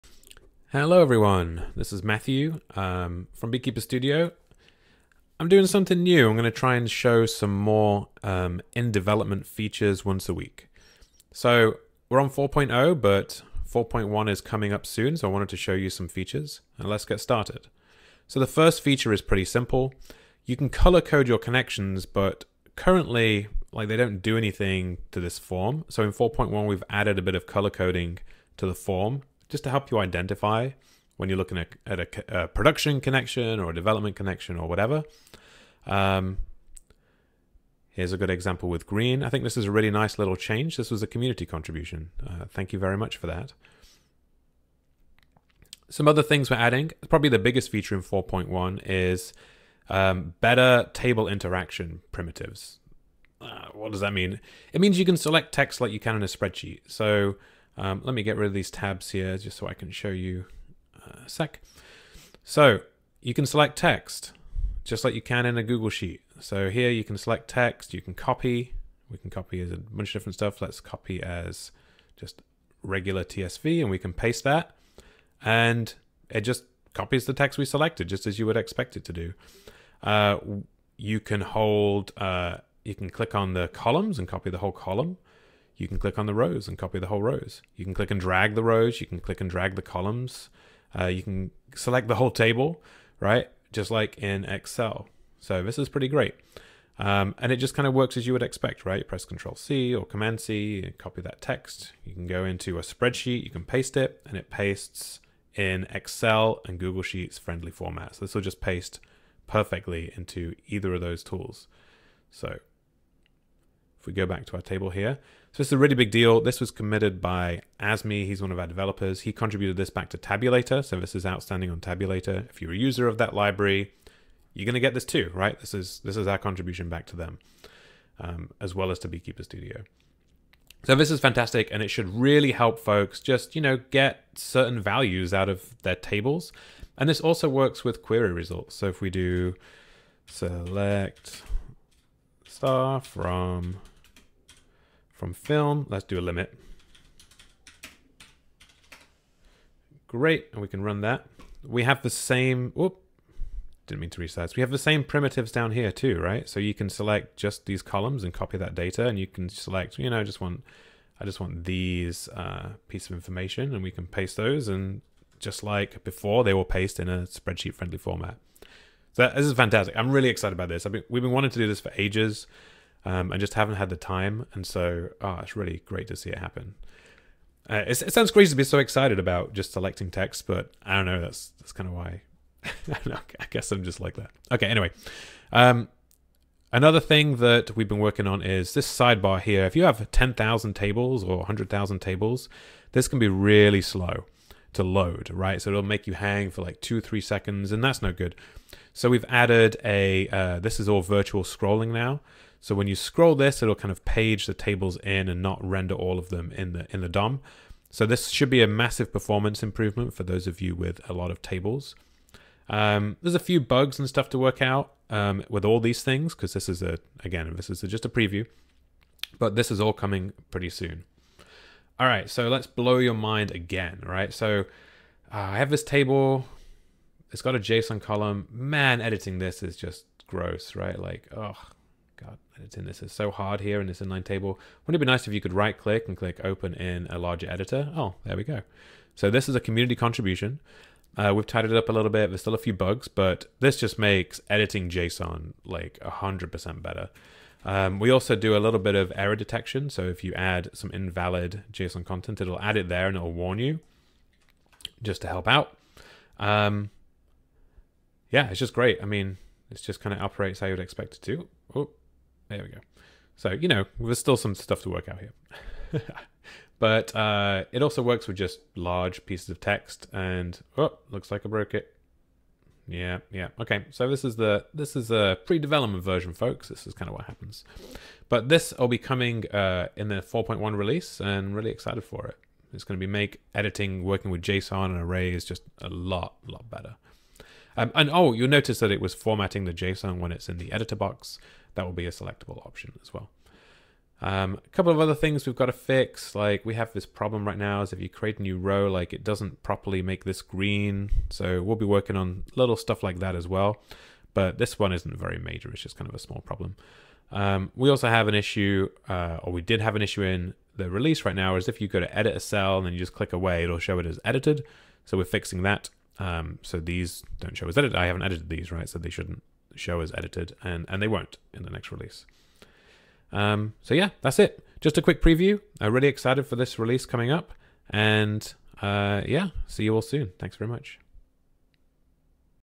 Upcoming feature preview - spreadsheet mode, large editor, performance improvements, and more
Hey folks,
This is Matthew, I'm trying to upload videos more regularly with fun or useful stuff happening at Beekeeper Studio.

This time: 4.1 release mini feature preview

- Edit JSON in your database in a pop-out editor
- Select database table data like a spreadsheet
- Better table loading performance
- Color coding in the connection form

Also - watch me break the app, live in the demo. All good fun.

Reminder: Beekeeper Studio is a SQL editor and database manager for Postgres, SQLite, MySQL, SQL Server, and more!